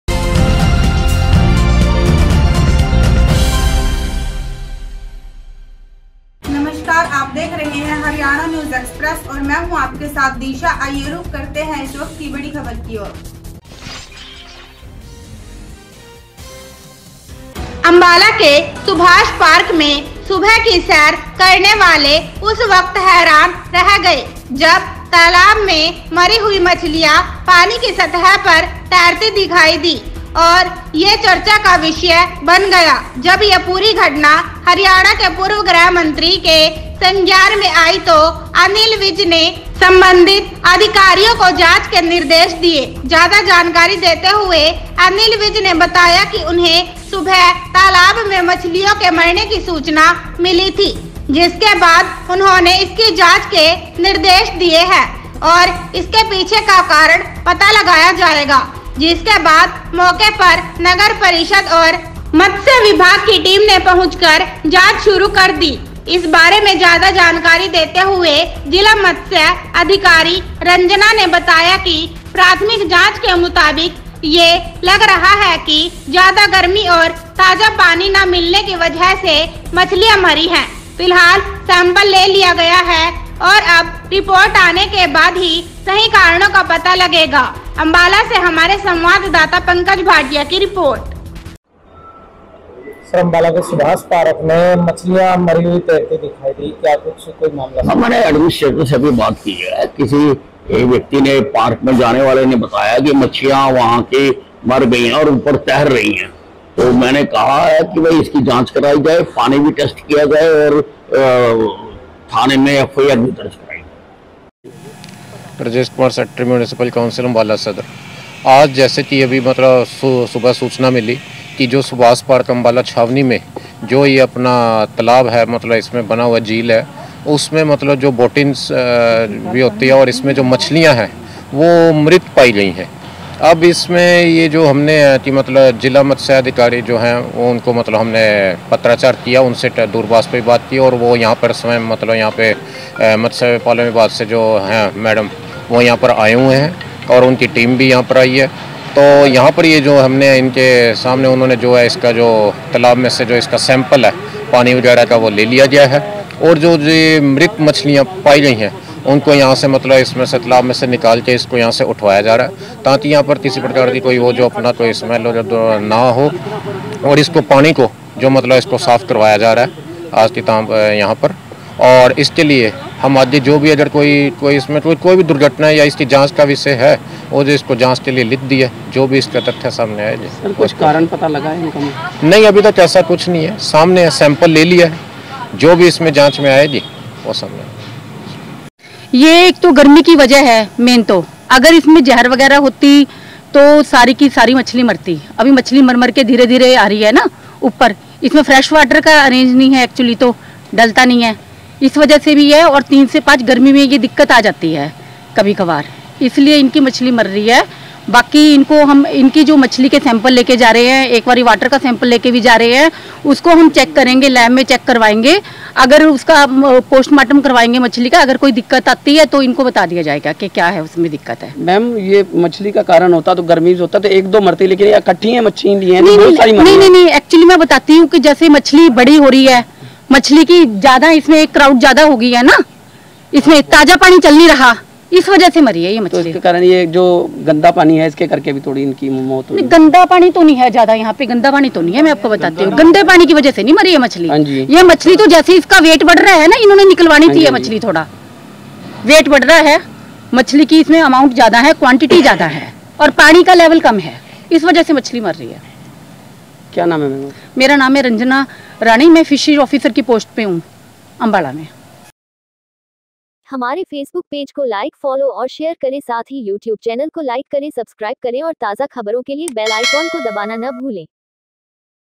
अम्बाला : सुभाष पार्क के तालाब में मरी हुई मछलियां दिखने से मचा हड़कंप
अम्बाला के सुभाष पार्क में सुबह की सैर करने वाले उस वक्त हैरान रह गए जब तालाब में मरी हुई मछलियां पानी की सतह पर तैरती दिखाई दी और ये चर्चा का विषय बन गया। जब ये पूरी घटना हरियाणा के पूर्व गृह मंत्री के संज्ञान में आई तो अनिल विज ने संबंधित अधिकारियों को जांच के निर्देश दिए। ज्यादा जानकारी देते हुए अनिल विज ने बताया कि उन्हें सुबह तालाब में मछलियों के मरने की सूचना मिली थी जिसके बाद उन्होंने इसकी जांच के निर्देश दिए हैं और इसके पीछे का कारण पता लगाया जाएगा। जिसके बाद मौके पर नगर परिषद और मत्स्य विभाग की टीम ने पहुंच कर जांच शुरू कर दी। इस बारे में ज्यादा जानकारी देते हुए जिला मत्स्य अधिकारी रंजना ने बताया की प्राथमिक जांच के मुताबिक ये लग रहा है कि ज्यादा गर्मी और ताजा पानी ना मिलने की वजह से मछलियां मरी है। फिलहाल सैंपल ले लिया गया है और अब रिपोर्ट आने के बाद ही सही कारणों का पता लगेगा।

अम्बाला छावनी में बना सुभाष पार्क बच्चों और बुजुर्गों की पसंद है हर वर्ग के लोगों को ध्यान में रखते हुए इसे बनाया गया है आज सुबह सुभाष पार्क में सैर करने आए लोग पार्क में बने तालाब को देख कर हक्के बक्के रह गए, दरअसल तालाब में मरी हुई मछलियां पानी की सतह पर दिखाई दी और ये बात चर्चा का विषय बन गई। इस घटना की सूचना पूर्व मंत्री विज को मिली जिसके बाद उन्होंने संबंधित अधिकारियों को जांच के निर्देश दिए, ज्यादा जानकारी देते हुए पूर्व गृह मंत्री विज ने बताया की उन्हें सुबह तालाब में मछलियों के मरने की सूचना मिली थी जिसके बाद उन्होंने इसकी जांच के निर्देश दिए है और इसके पीछे का कारण पता लगाया जाएगा।

मौके पर पहुंची नगर परिषद की टीम में जांच शुरू की और इस बारे में ज्यादा जानकारी देते हुए नगर परिषद सचिव राजेश ने बताया कि सुबह उन्हें सूचना मिली थी सुभाष पार्क में बने तालाब में मछलियां मरी हुई तैर रही है, जिसके बाद उन्होंने जिला मत्स्य विभाग को संपर्क किया और वे मौके पर पहुंचे उन्होंने सभी सैंपल लिए और मछलियों को पोस्टमार्टम के लिए लेकर गए। फिलहाल रिपोर्ट आने के बाद ही असली कारणों का पता लगाया जाएगा। इस बारे में ज्यादा जानकारी देते हुए जिला मत्स्य अधिकारी रंजना ने बताया की प्राथमिक जांच के अनुसार ये लग रहा है कि ज्यादा गर्मी और ताजा पानी न मिलने की वजह से मछलियां मरी है। फिलहाल सैंपल ले लिया गया है और अब रिपोर्ट आने के बाद ही सही कारणों का पता लगेगा।